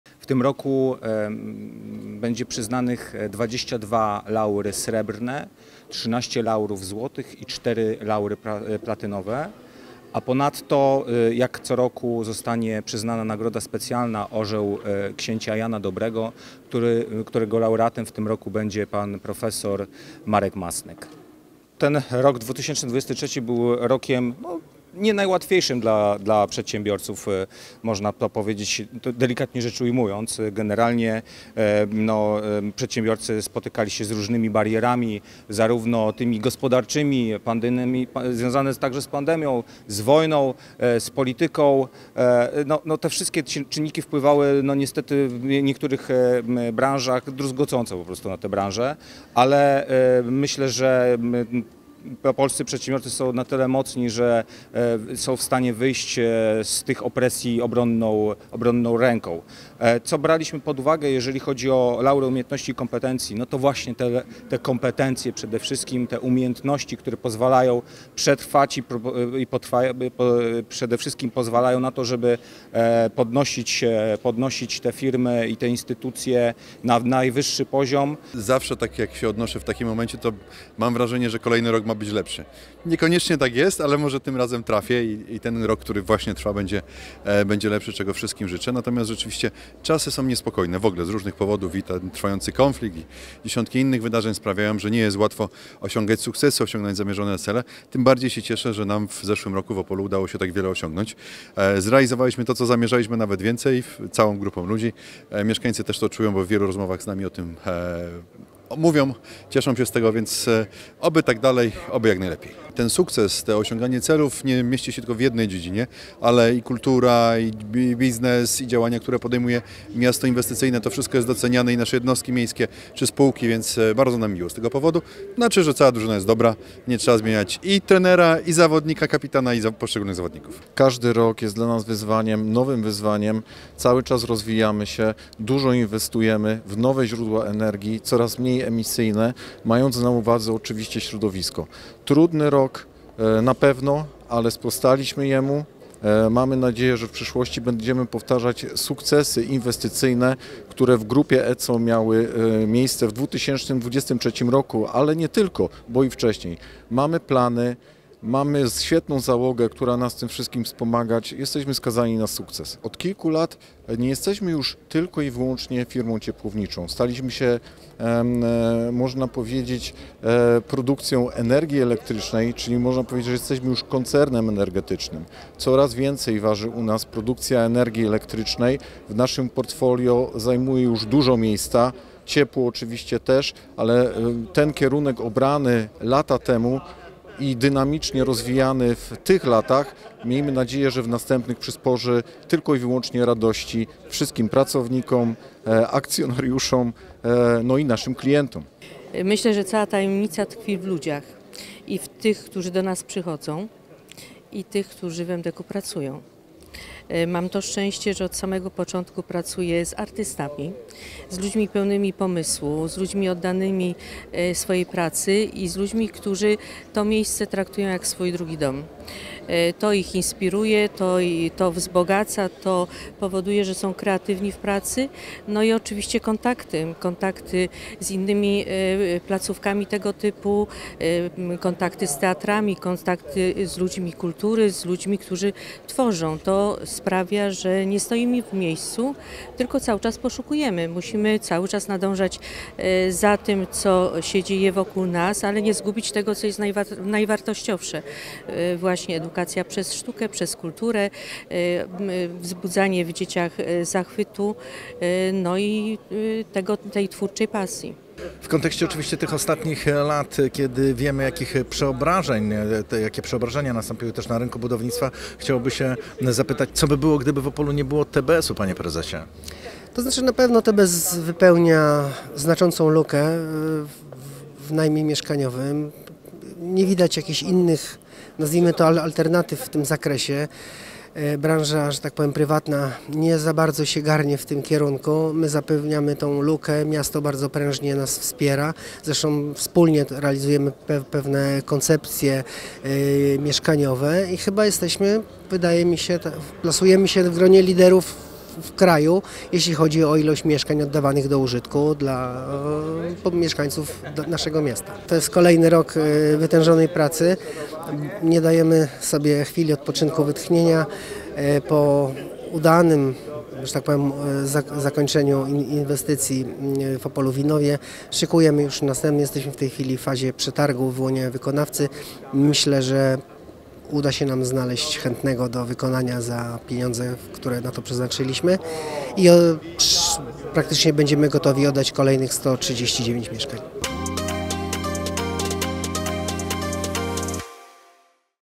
Laury Umiejętności i Kompetencji za 2023 rok
Już po raz 24. wręczono Laury Umiejętności i Kompetencji Opolskiej Izby Gospodarczej. W sumie przyznano 40 wyróżnień. W tym cztery platynowe, trzynaście złotych oraz dwadzieścia dwa srebrne. Nagrodę specjalną, Orła Księcia Jana Dobrego wręczono rektorowi UO prof. dr. hab. Markowi Masnykowi.  Wśród wyróżnionych znalazła się m.in. Elżbieta Marciniszyn wieloletnia dyrektorka Młodzieżowego Domu Kultury (Srebrny Laur w kategorii „Kultura i sport”), Opolskie Towarzystwo Budownictwa Społecznego (Złoty Laur w kategorii „Załoga – wspólny sukces”) oraz Ewa Nowak- Kübler w kategorii Osoba lub instytucja wspierająca rozwój gospodarki rynkowej lub edukująca na potrzeby firm – Dyrektor Zespołu Szkół Ekonomicznych im. Gen. Stefana Roweckiego „ Grota” w Opolu. Z kolei platynowym laurem uhonorowano miejską spółkę ECO S.